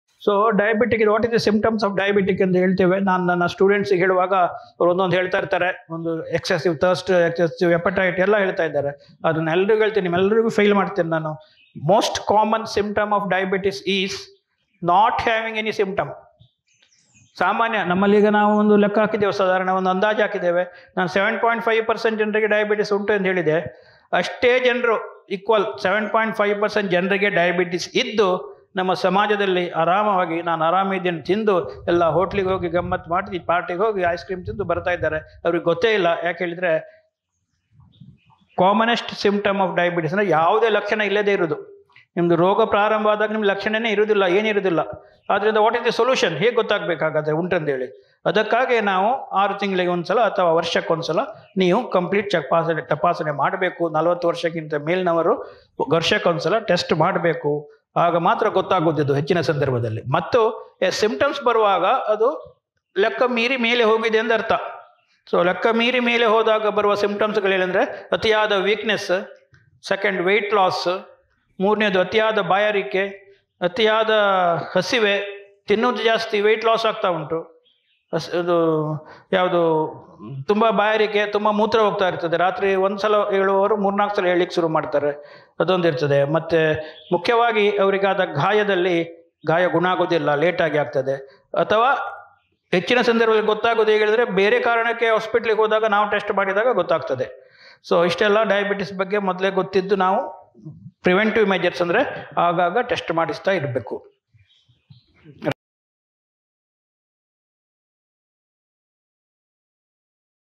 ಮಧುಮೇಹ - ರೋಗಲಕ್ಷಣಗಳಿಲ್ಲದ ಖಾಯಿಲೆ ? | How Unhealthy Lifestyle Can Cause Prediabetes and Diabetes
ಮಧುಮೇಹ -  ರೋಗಲಕ್ಷಣಗಳಿಲ್ಲದ ಖಾಯಿಲೆ ? ಇದರ ಬಗ್ಗೆ Dr Ramachandra Joishy ಅವರು ನೀಡಿದ ಸಂಕ್ಷಿಪ್ತವಾದ ಮಾಹಿತಿಯನ್ನು ಉದಯವಾಣಿ ಬಳಗವು ನಿಮ್ಮ ಮುಂದೆ ಪ್ರಸ್ತುತ ಪಡಿಸುತ್ತಿದೆ.

For more..